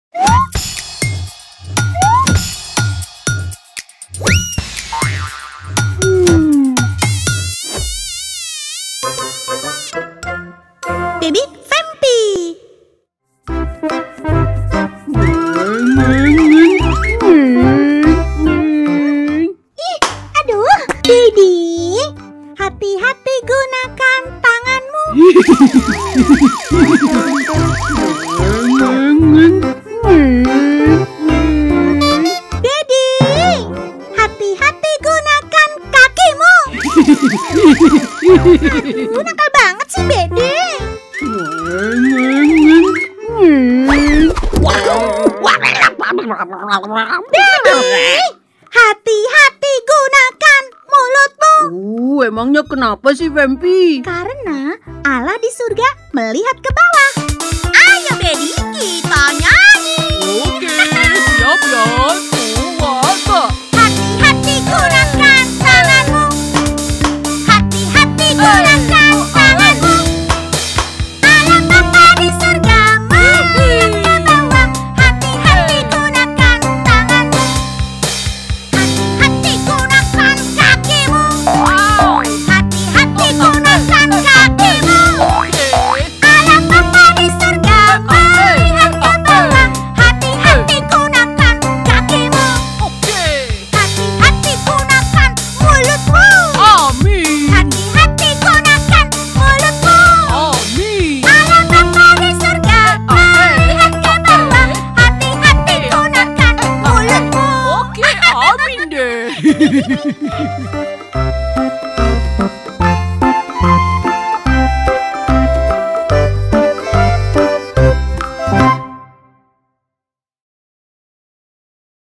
BABY VAMPY - HATI-HATI
Baddy, vampir yang usil, malas, dan tidak pernah bisa diam kembali membuat kegaduhan. Akibat kemalasannya ini yang bikin gaduh. Untung ada Vampy yang siap sedia memberi tau Baddy supaya lebih baik.

Saksikan keseruan cerita Vampy dan Baddy dalam Pendidikan Karakter Anak: Baby Vampy dalam judul 'Hati-hati'. Tayang perdana 27 November 2022.

Pendidikan Karakter Anak: Baby Vampy adalah segmen terbaru di UPPKomunikasi KAS. Segmen ini untuk menemani anak-anak supaya makin gembira dan tumbuh dengan baik.

Berkah Dalem